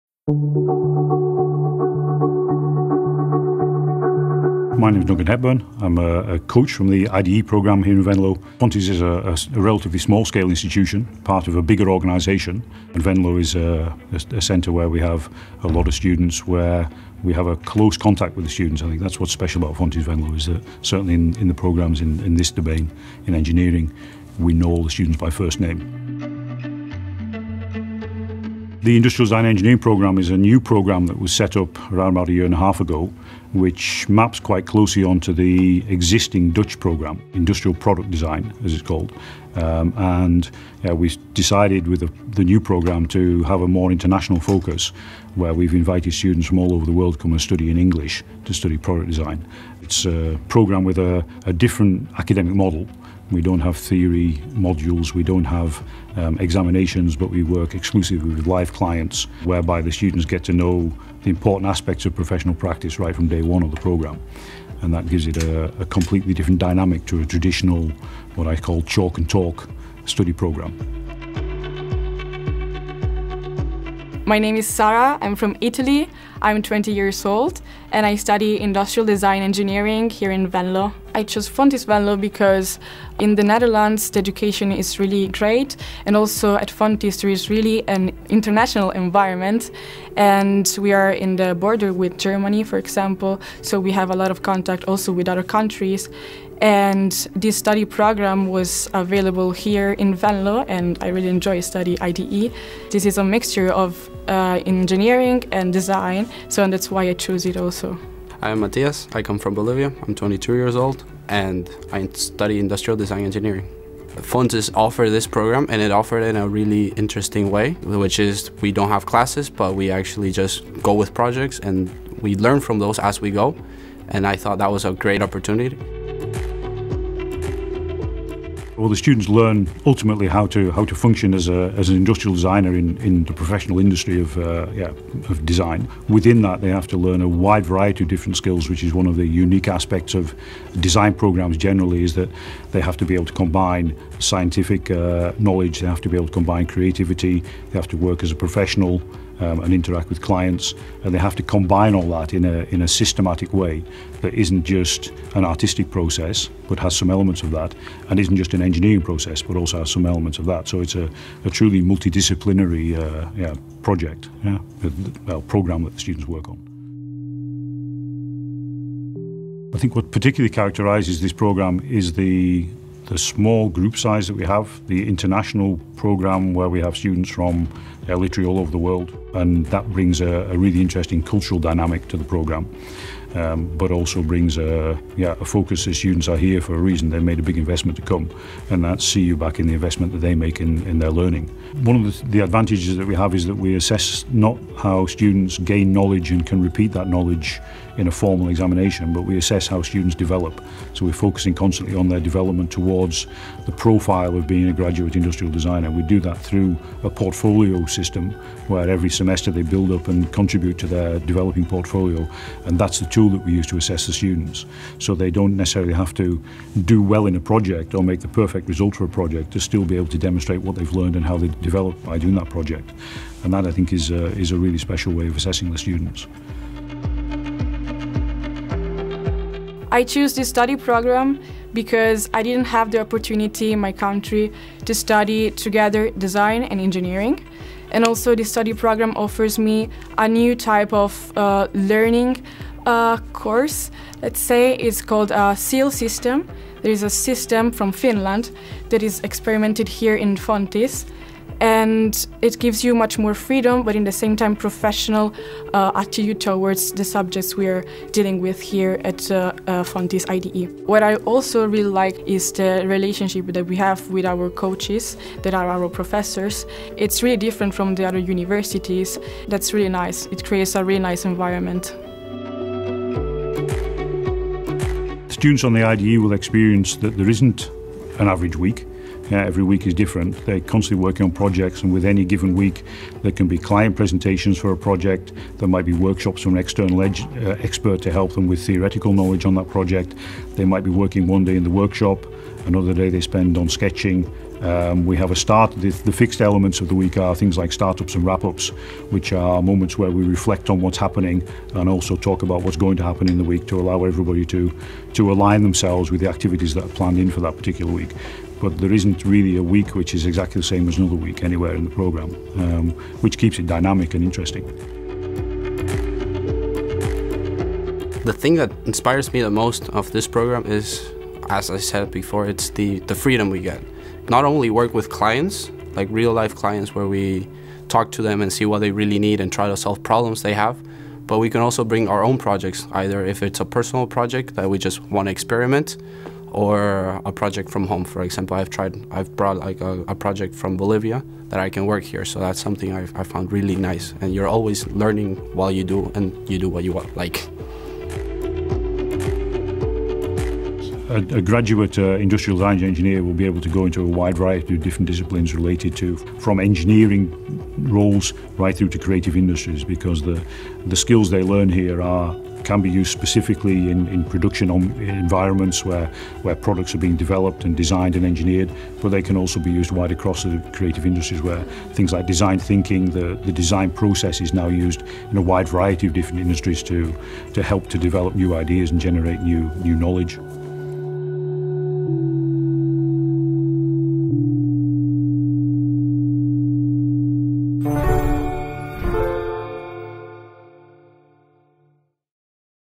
Get to know the Industrial Design Engineering study programme at Fontys Venlo
Get to know the Industrial Design Engineering study programme at Fontys Venlo and learn more about the content of the study programme, the subjects you will get and find out what kind of professions you can practice after graduating!